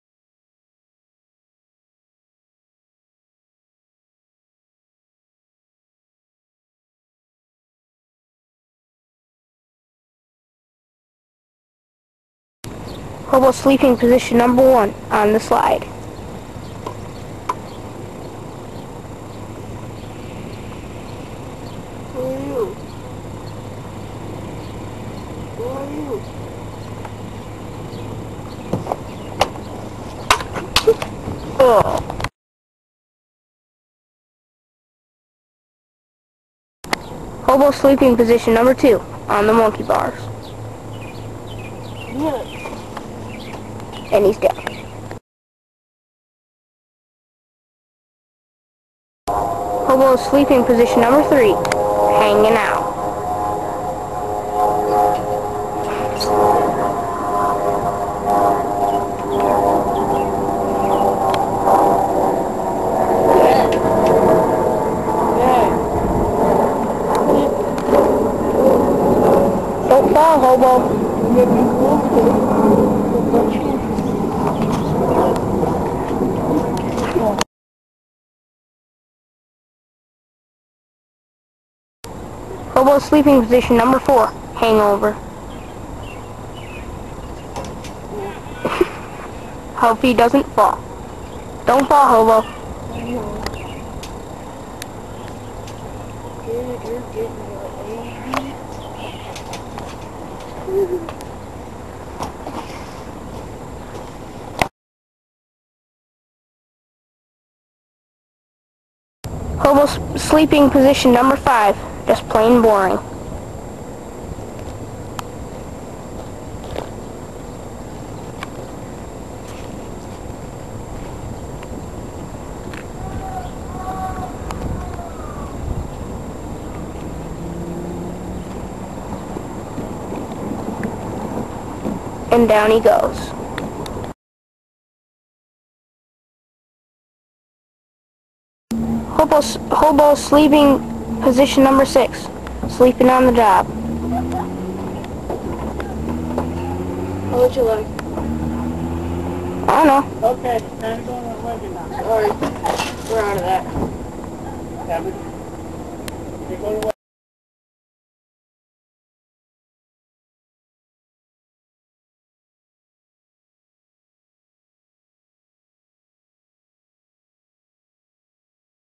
The Life of a Hobo
Sleeping Positions-The many sleeping positions of hobos.